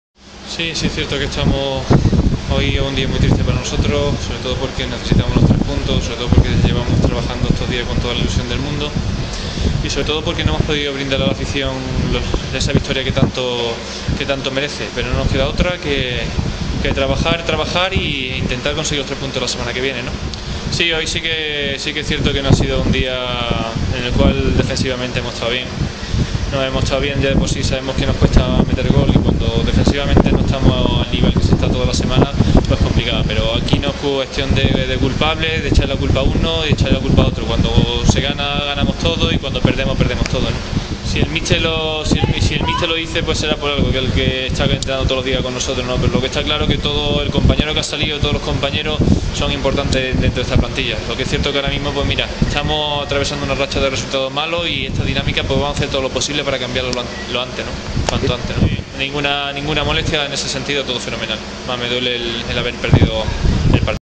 [2010-2011] [J25] [2ªB Grupo I] CD Badajoz SAD 1 - 2 CP Cacereño
[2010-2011] [Jornada 25] Palabras de Aloisio tras el CD Badajoz SAD 1 - 2 CP Cacereño. [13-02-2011.